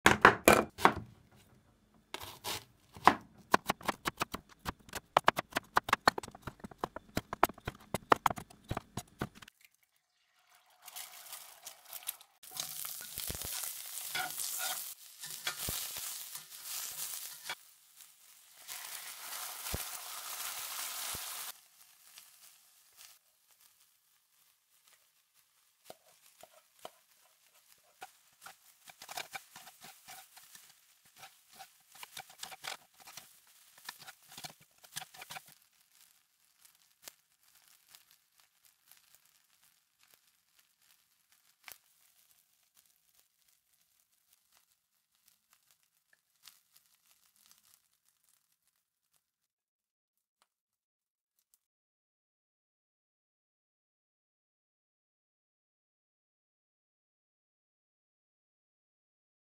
Sardine Fried Rice In 1 Minute
Cooking one minute at a time.
Today I'm cooking Sardine Fried Rice In 1 Minute using the Tower Vortx.